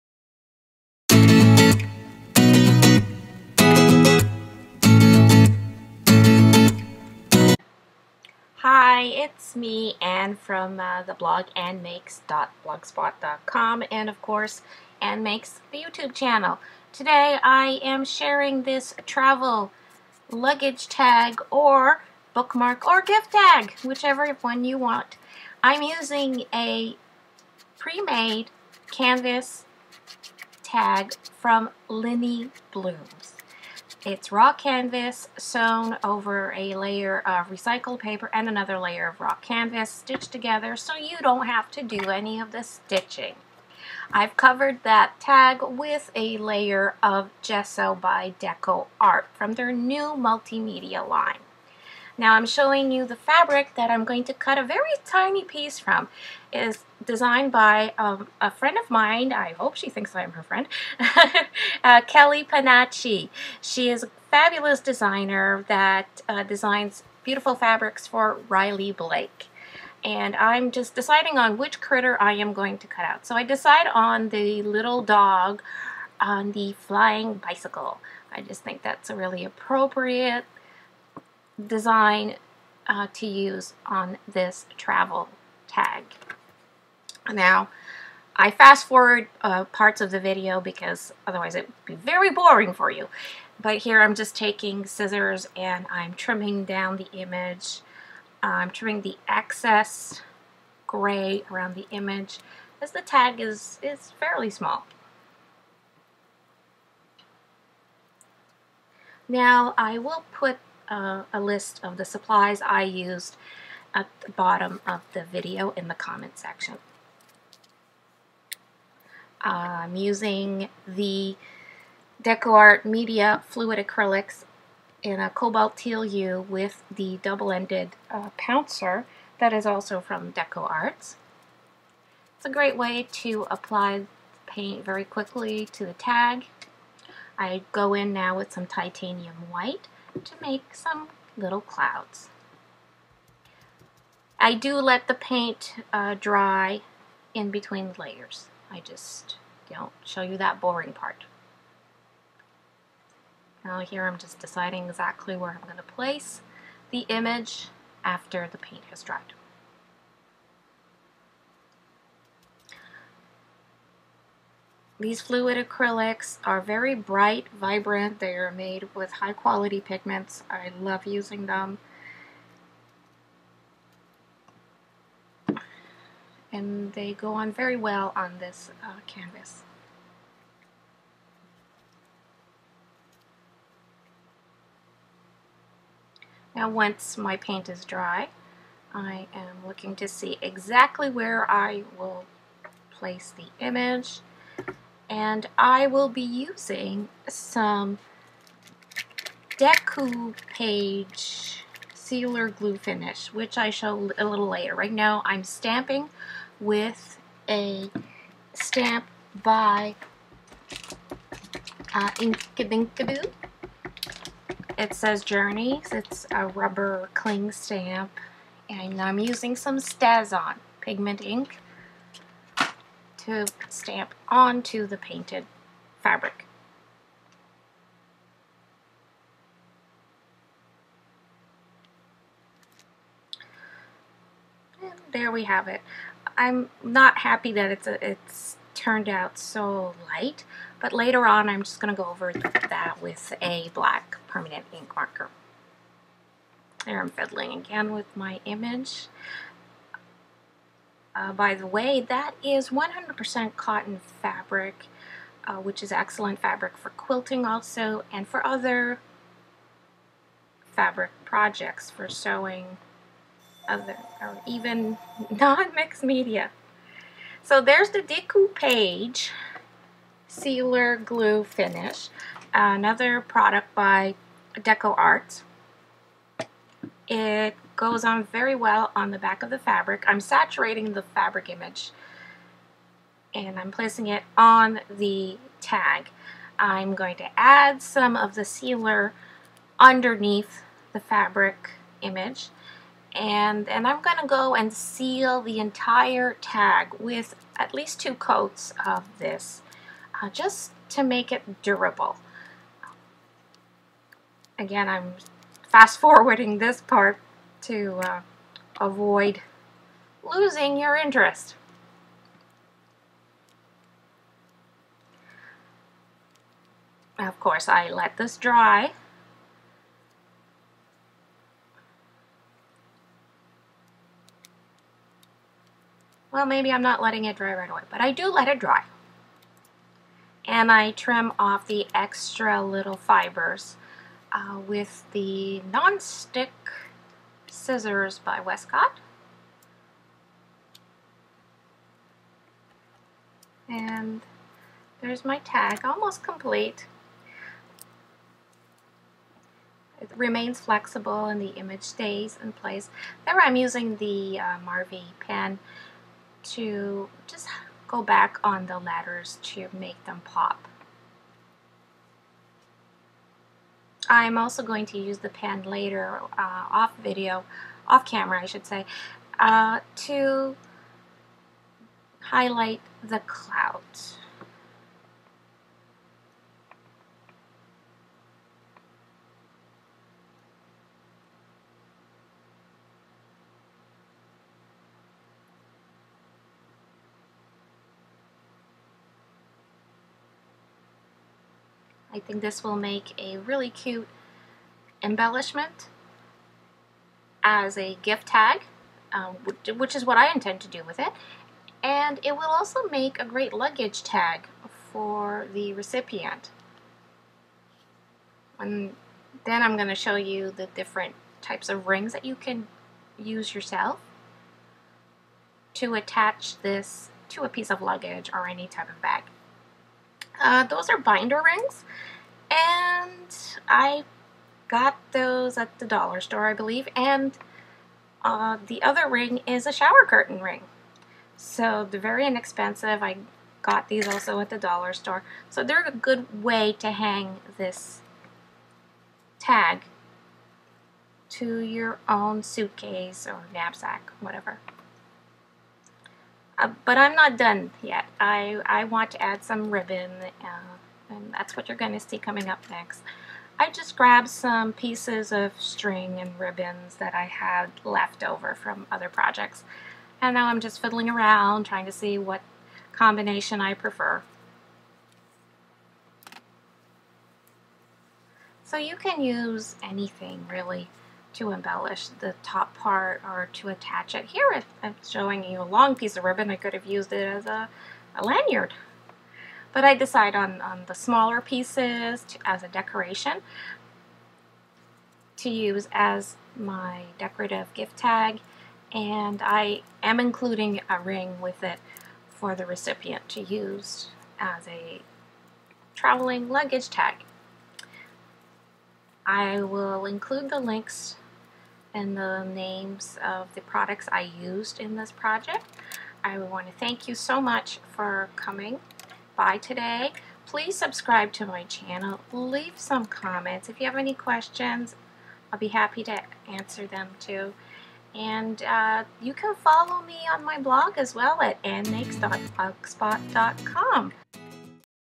Lovesummerart Travel Tag How to
This video is about the Lovesummerart Travel Tag How to. I show you how I turn a raw canvas tag into aembellishment that can be used just as is, a book mark, to give with a gift, or to use to label your luggage.

lovesummerart July 17-20 is a collaborative effort with many YouTube artists and crafters showing great content within the same theme. We hope to make this a monthly event.

(stencil "Daisy Chain" 6"x8" Americana; DecoArt® Media Fluid Acrylics in Cobalt Teal Hue, and Titanium White; Dual sided Paint Pouncers; DecoArt® Media Gesso in White; Americana Decou-Page sealer-glue-finish; and American Applicator tool.
Kelly Panacci Inc. "Little Flyers" Riley Blake fabrics
Westcott Titanium non stick scissors

Other supplies used:  StazOn Jet black Solvent Ink Pad by Tsukineko.
Black Permanent Ink Pen by Marvy® . Rubber Stamps That Cling by Inkadinkado® "Delivery Labels",  and binder rings, shower curtain rings from Dollarama.  Note fabric and string trims are from my stash of scraps.